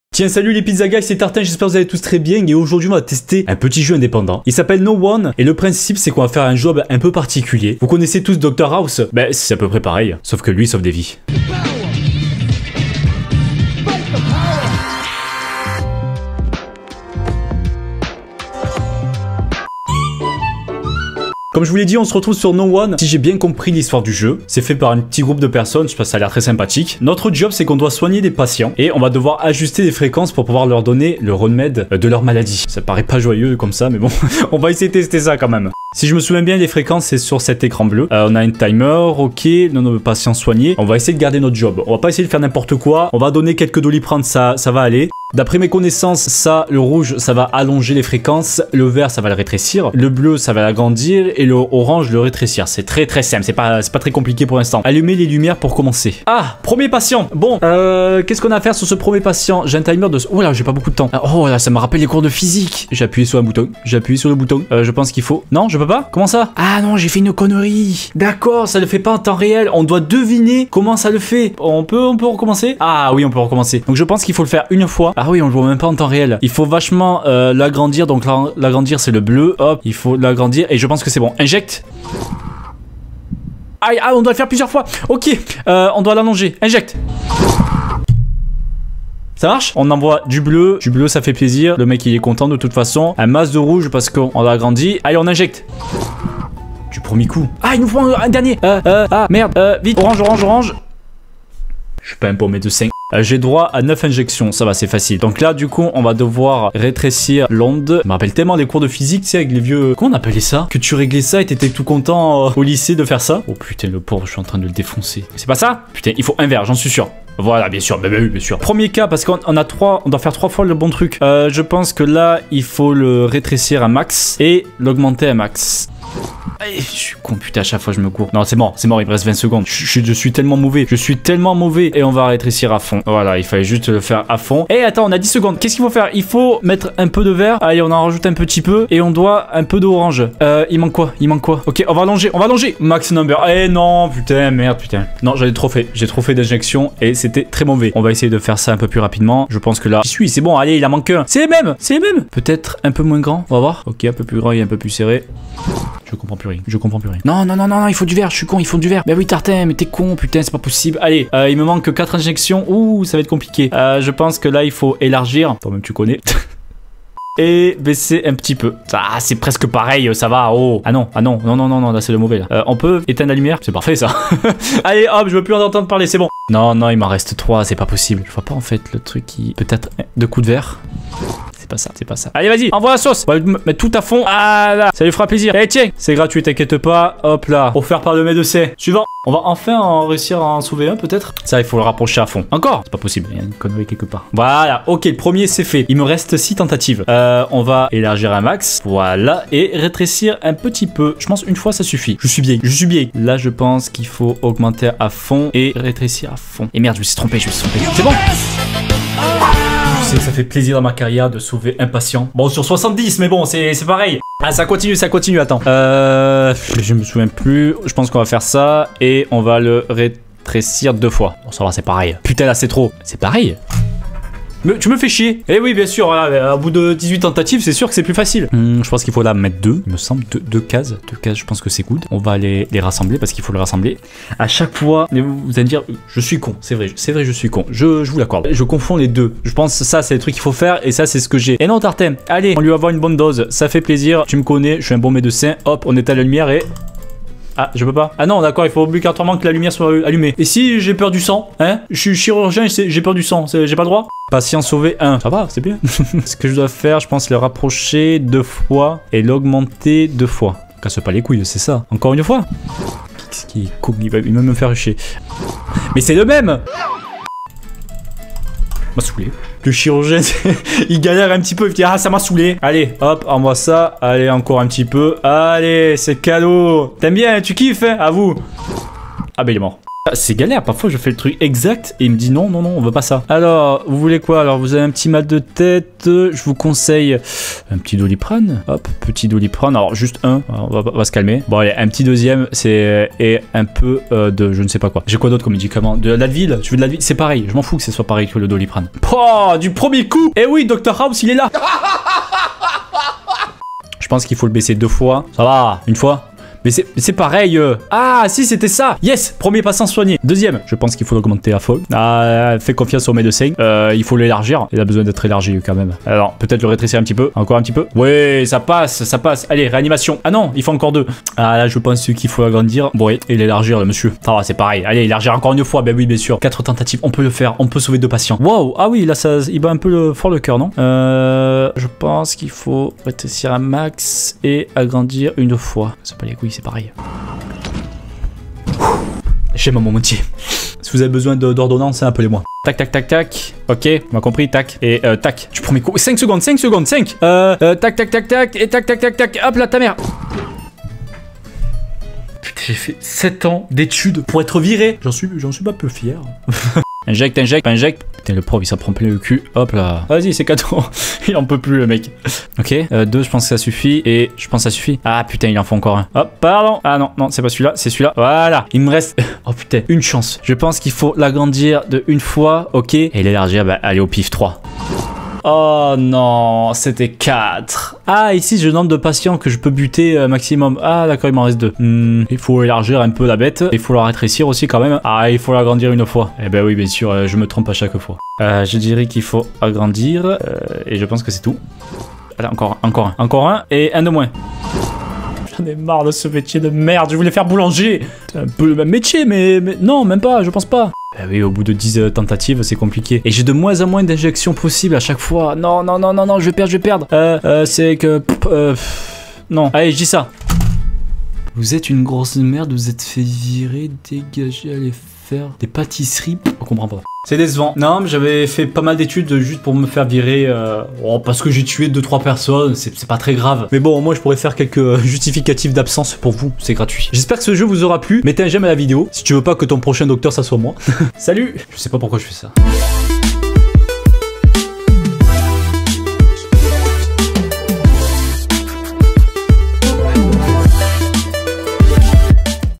JE SUIS LE PIRE MEDECIN DU MONDE !
Tartinex se met dans la peau d'un médecin, qui veut une consultation ?